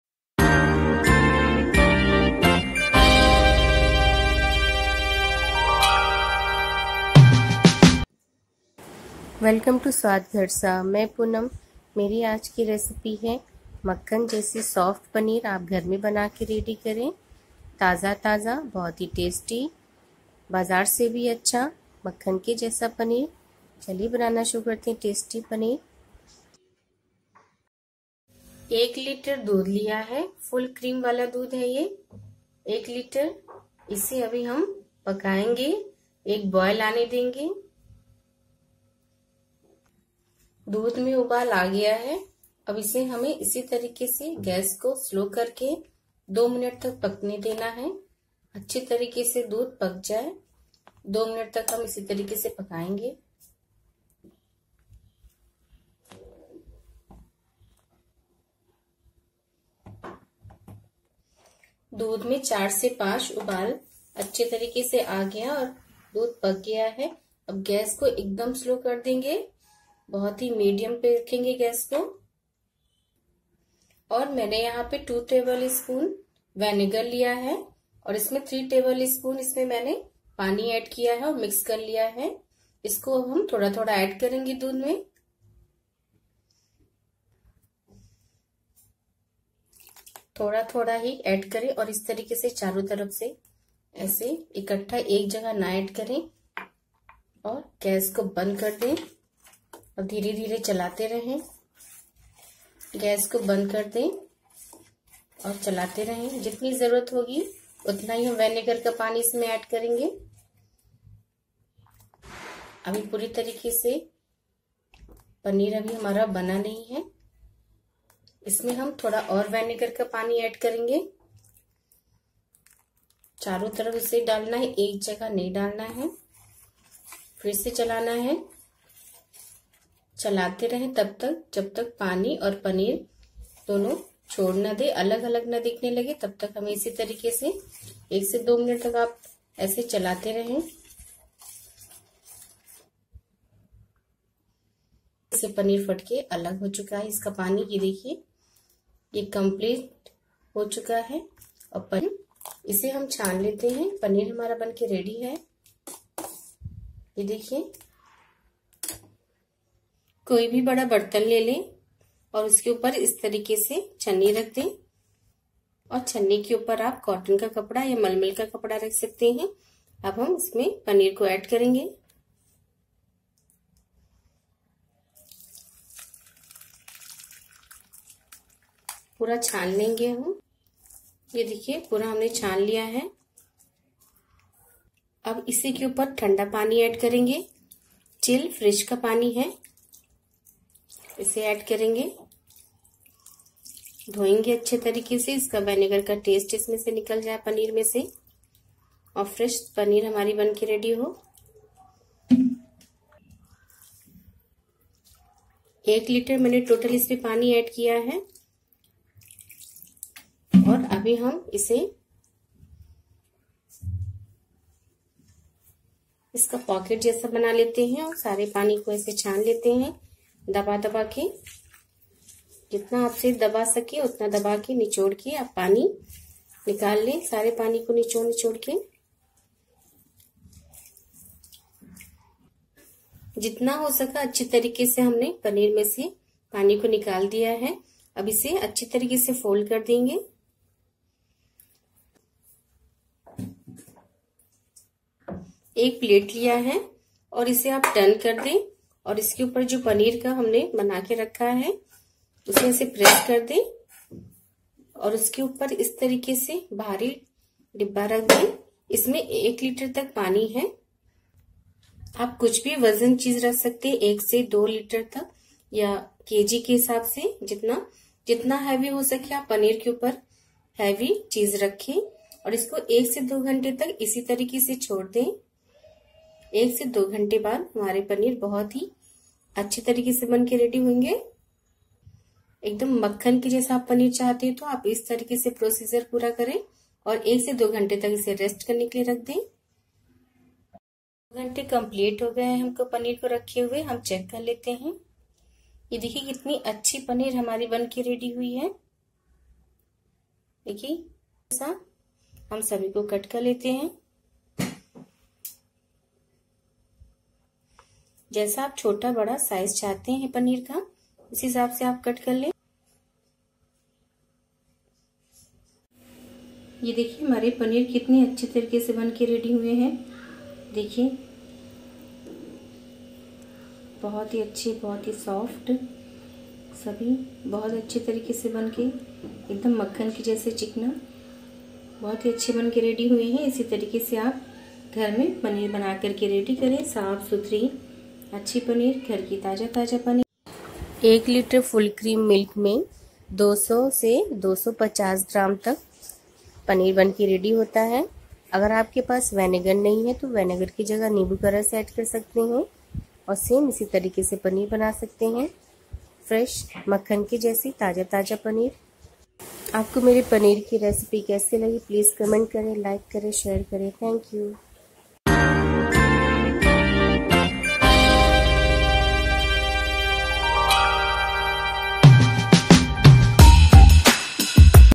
पनीर कैसे बनाए | Home Made Paneer Recipe Swad Ghar Sa
Very tasty & soft. Paneer...( डेरी में  मिलने वाले पनीर से अच्छा आप घर में पनीर बना के  तैयार करे .) .सॉफ्ट पनीर(   tasty paneer )

Paneer पनीर मक्खन पनीर घर# में बनाये